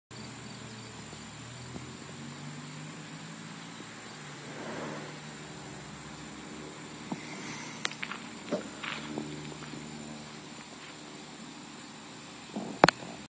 96 Mustang GT sequential taillights (teaser)
Quick teaser of my hazards on my 96 GT with the American Muscle splice in kit